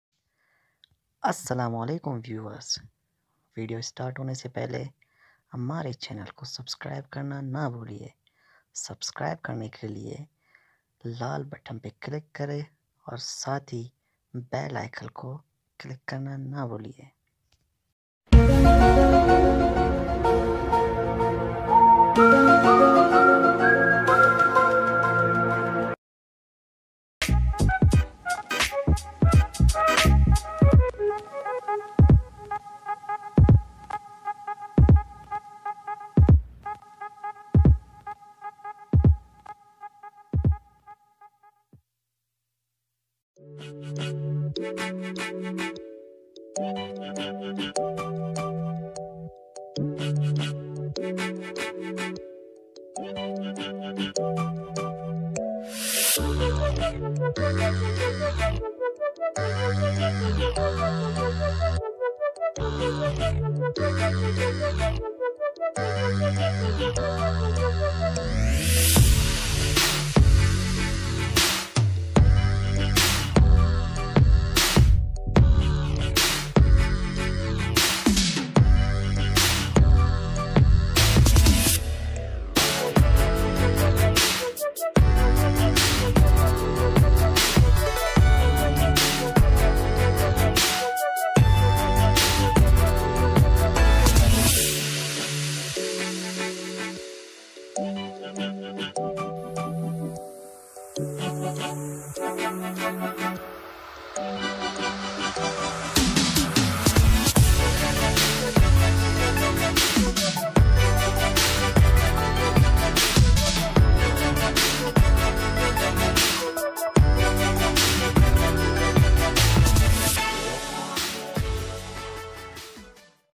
Raheel Shareef 2019 Main Pakistan Ka Naya Saddar راحیل شریف
It is prediction by rohani peshwa that raheel shareef will become next President of Pakistan. let see how much truth is this prediction.